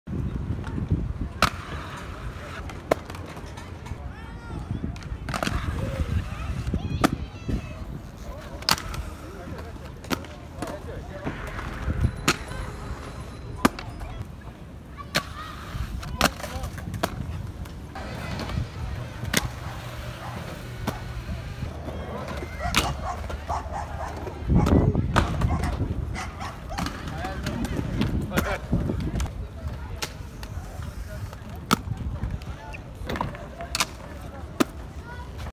Kalamış Skatepark
Kalamış skatepark da paten ile 1 gün

Agresif   Paten Hakkında Herşey
Snowboard
Kayak
Kaykay
BMX
Longboard
Freeboard